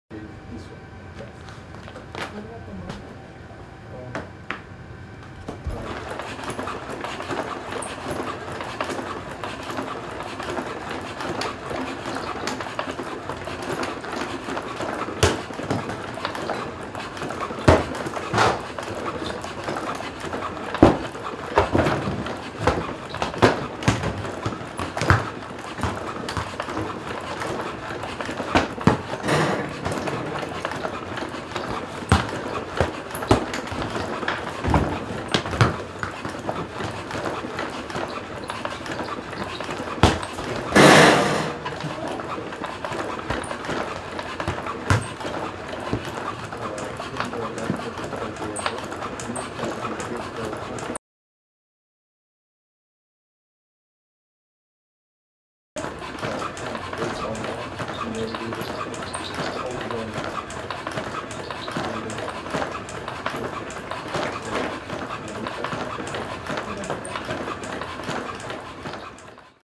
Mercury, Passive-ankle biped robot (from UT Austin HCRLab)
Mercury is a passive-ankle biped robot that can maintain its balance by continuously taking recovery stepping.